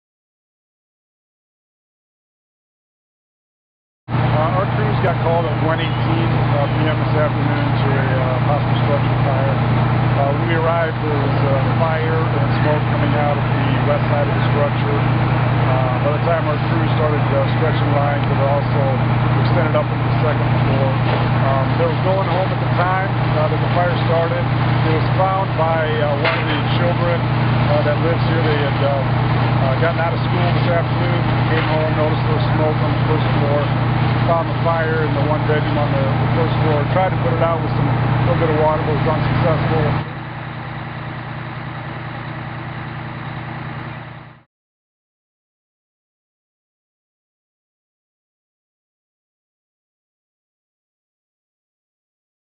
Rock Island House Fire
Rock Island firefighters responded to a house fire at 1:18 p.m. Monday at 604 19th Avenue. Smoke was billowing from the 2-story structure when officers arrived on the scene. One resident discovered the fire but was unable extinguish the fire before exiting the structure. No further details were available at the scene.